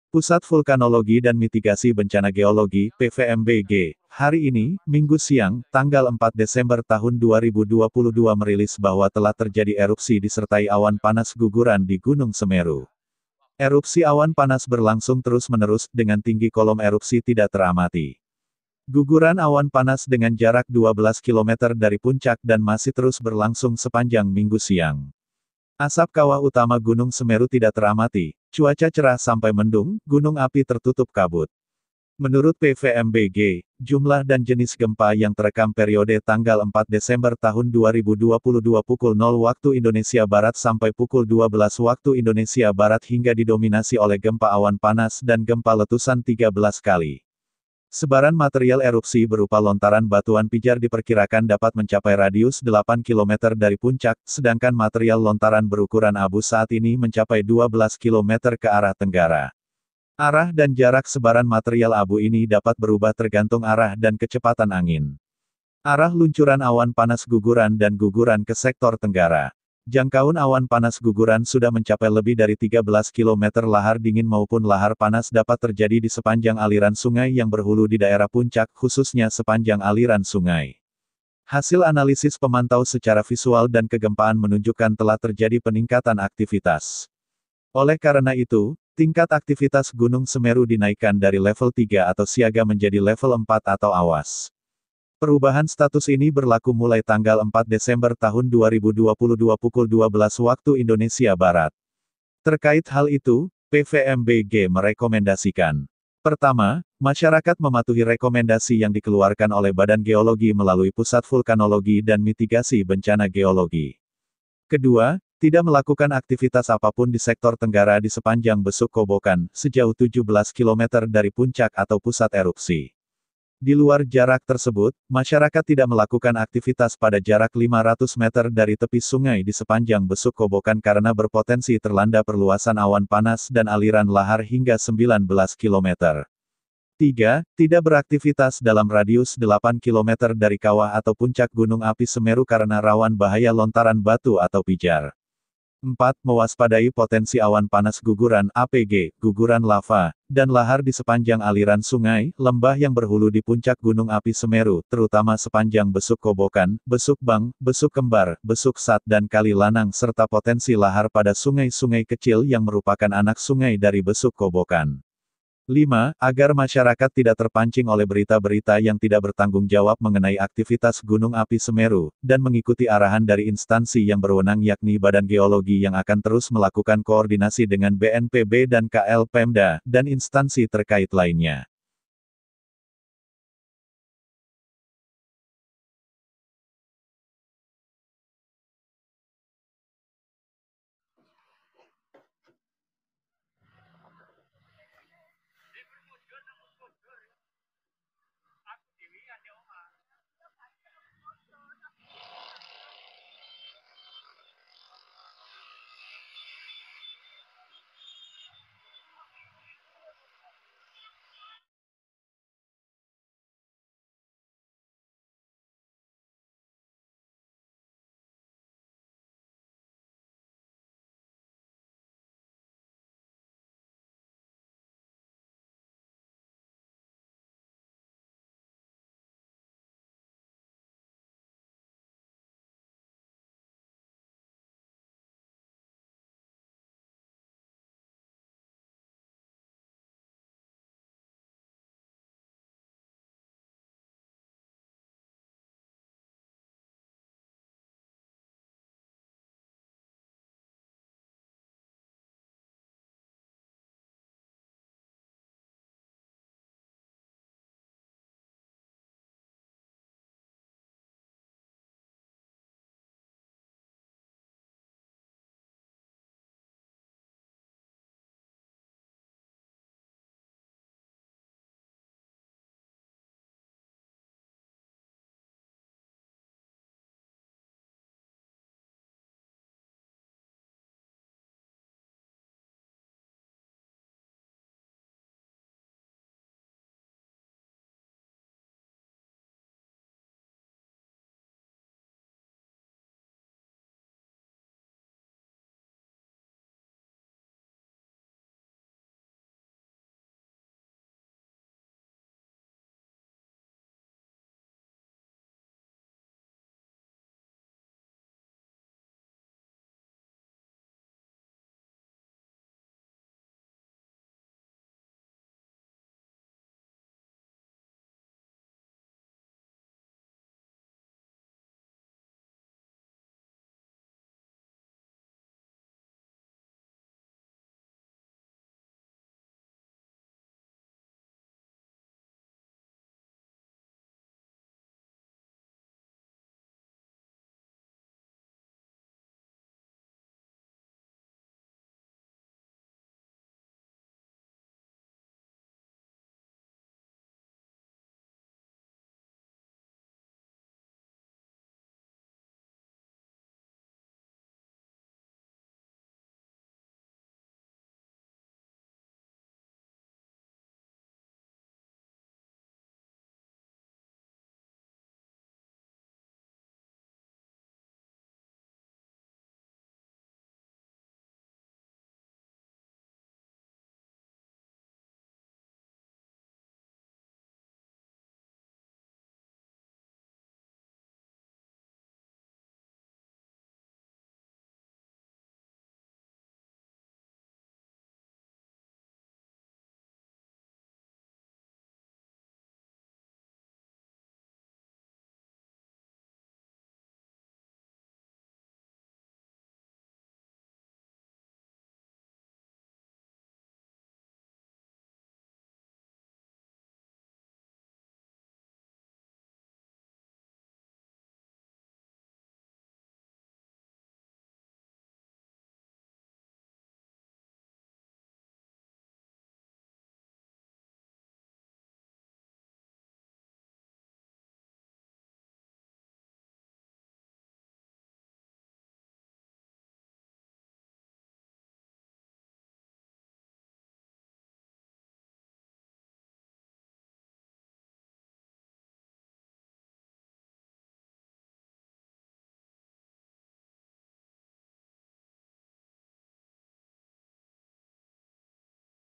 Ngeri dan Mencekam! Gunung Semeru Muntahkan Awan Panas Hingga 17 Kilometer
Ngeri dan mencekam. Itulah yang terjadi sesaat setelah Gunung Semeru di Lumajang, Jawa Timur, memuntahkan awan panas pada Minggu siang, 4  Desember 2022.  Sebelumnya Gunung Semeru memang sudah berstatus Waspada. Namun, dengan muntahan awan panas dalam jumlah besar pada Minggu siang itu, Badan Geologi kemudian meningkatkan status Semeru menjadi Awas.

Pusat Vulkanologi dan Mitigasi Bencana Geologi (PVMBG) hari ini, Minggu siang , 4 Desember 2022 merilis bahwa telah terjadi erupsi yang disertai Awan Panas Guguran masih berlangsung menerus, dengan tinggi kolom erupsi tidak teramati.

Teramati awan panas guguran dengan jarak 12 kilometer dari puncak dan masih terus berlangsung.

Asap kawah utama tidak teramati, cuaca cerah sampai mendung, gunung api tertutup kabut. Angin tenang ke selatan, tenggara, barat, barat laut, barat daya, suhu 22-28 derajat celcius.

Jumlah dan jenis gempa yang terekam periode 4 Desember 2022 pukul  00.00 WIB sampai 12.00 Waktu Indonesia Barat hingga didominasi oleh Gempa Awan Panas dan gempa letusan 13 kali.  Amplitudo awan panas terekam 40 mm dan masih berlangsung hingga saat ini.

Sebaran material erupsi berupa lontaran batuan pijar diperkirakan dapat mencapai radius 8 kilometer dari puncak, sedangkan material lontaran berukuran abu saat ini mencapai 12 kilo meter ke arah tenggara. Arah dan jarak sebaran material abu ini dapat berubah tergantung arah dan kecepatan angin.

Arah luncuran awan panas guguran dan guguran ke sektor tenggara dan selatan dari puncak,  Jangkaun awan panas guguran sudah mencapai lebih dari 13 Kilo meter Lahar dingin maupun lahar panas dapat terjadi di sepanjang aliran sungai yang berhulu di daerah puncak, khususnya sepanjang aliran sungai.

Hasil analisis pemantau secara visual dan kegempaan menunjukan terjadi peningkatan aktivitas, dan dinilai tingkat aktivitas G. Semeru dinaikkan dari level 3 atau Siaga menjadi Level 4 atau Awas. Perubahan status ini berlaku mulai 4 Desember 2022 pukul 12.00 WIB.

PVMBG merekeomendasikan:

Pertama, masyarakat mematuhi rekomendasi yang dikeluarkan oleh Badan Geologi melalui Pusat Vulkanologi dan Mitigasi Bencana Geologi.

Kedua, tidak melakukan aktivitas apapun di sektor tenggara di sepanjang Besuk Kobokan, sejauh 17 km dari puncak atau pusat erupsi. Di luar jarak tersebut, masyarakat tidak melakukan aktivitas pada jarak 500 meter dari tepi sungai  di sepanjang Besuk Kobokan karena berpotensi terlanda perluasan awan panas dan aliran lahar hingga 19 km.

Tiga, tidak beraktivitas dalam radius 8 Km dari kawah/puncak Gunung Api Semeru karena rawan terhadap bahaya lontaran batu atau pijar.

Empat, ewaspadai potensi awan panas guguran (APG), guguran lava, dan lahar di sepanjang aliran sungai/lembah yang berhulu di puncak Gunung Api Semeru, terutama sepanjang Besuk Kobokan, Besuk Bang, Besuk Kembar, Besuk Sat dan Kali Lanang serta potensi lahar pada sungai-sungai kecil yang merupakan anak sungai dari Besuk Kobokan.

Lima, agar masyarakat tidak terpancing oleh berita-berita yang tidak bertanggungjawab mengenai aktivitas Gunung Api Semeru,dan mengikuti arahan dari Instansi yang berwenang yakni Badan Geologi yang akan terus melakukan koordinasi dengan BNPB dan K/L, Pemda, dan instansi terkait lainnya.

Gunungapi Semeru secara administratif terletak  dalam dua kabupaten, yaitu  Kabupaten Lumajang dan Kabupaten Malang, Provinsi Jawa Timur. Tinggi puncak Gunung Semeru mencapai 3.676 meter dari permukaan laut.
Gunungapi Semeru dipantau secara visual dan instrumental dari 2 Pos Pengamatan Gunungapi (PGA) yang berada di Desa Sumber Wuluh, Kecamatan Candipuro, Kabupaten Lumajang dan di Desa Agrosuko, Kecamatan Ampelgading, Kabupaten Malang.

Tingkat aktivitas Gunungapi Semeru pada saat ini adalah  Level III (Siaga) sejak 16 Desember 2021.